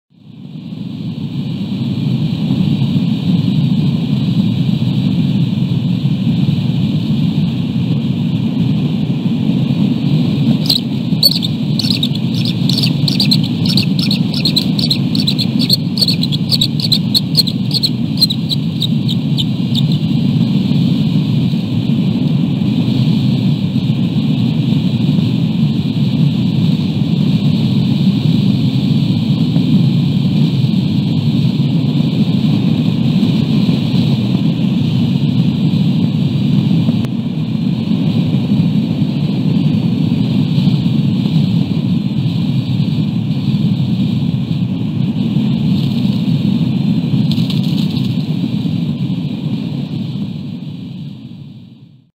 WE - Bonding on the Rock
K-01/Superman & K-91/Thunder unite their bond on their nest rock in preparation for the upcoming nesting season.